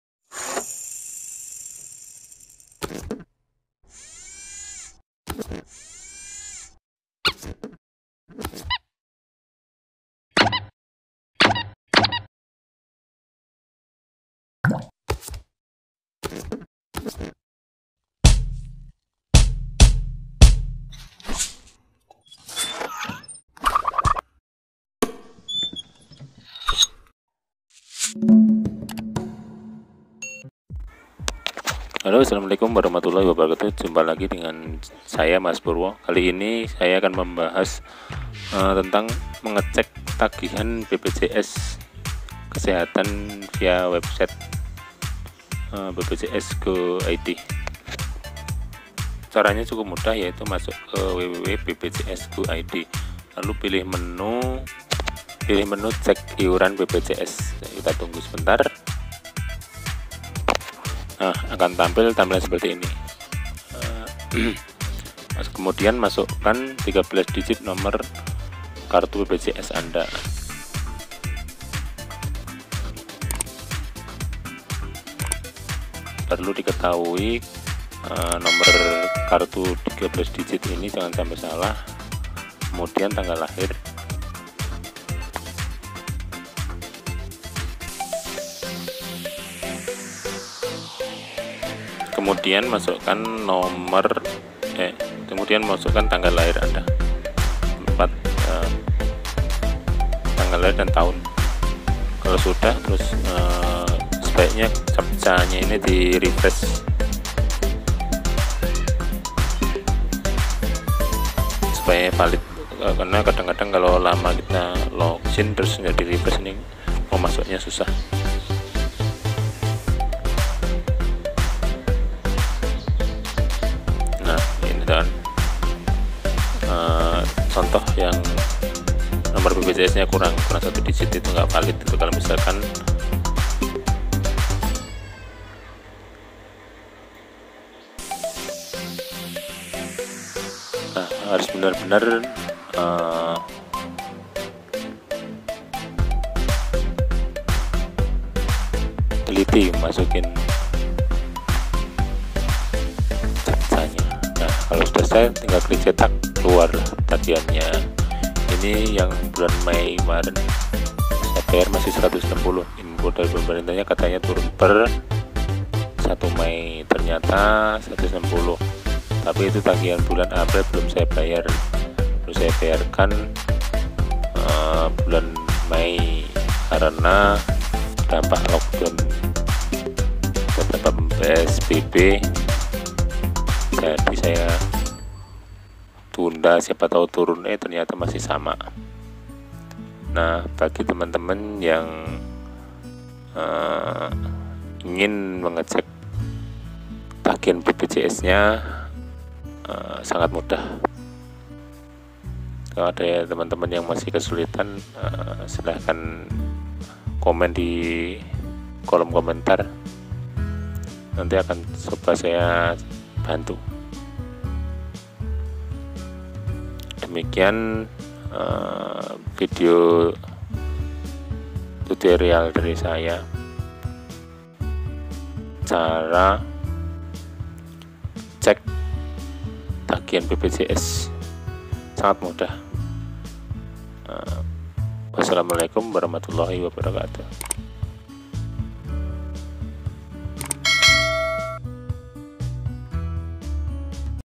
CEK TAGIHAN BPJS KESEHATAN BELUM TURUN
Vidio ini membahas tentang cara cek tagihan BPJS kesehatan, bagi anda yg ingin cek tagihan BPJS sebelum bayar ini bisa jadi reverensi